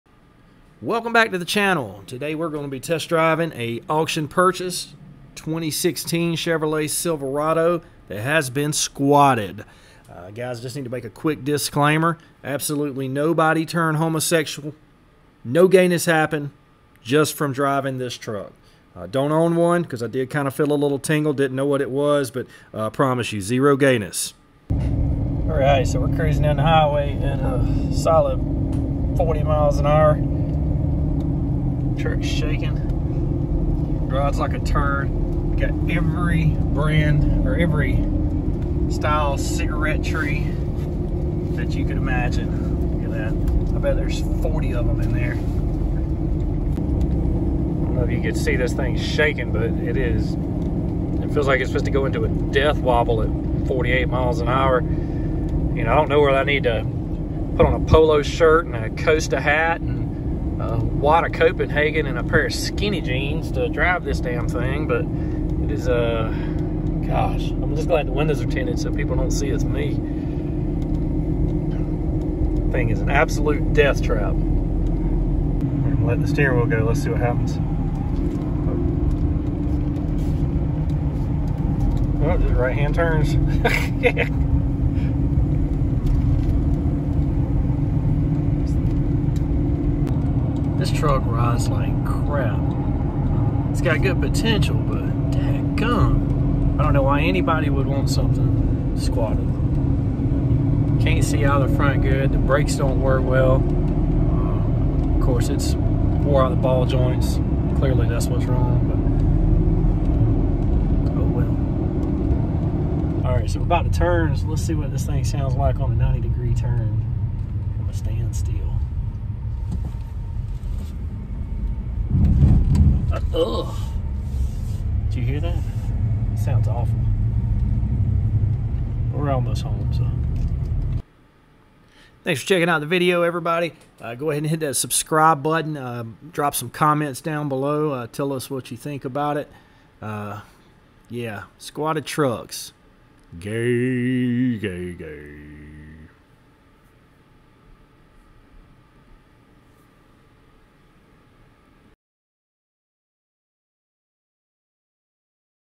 squatted Silverado
first time test driving a squatted truck! 2016 chevy Silverado with about 10" of lift on the front and dragging it's butt behind it.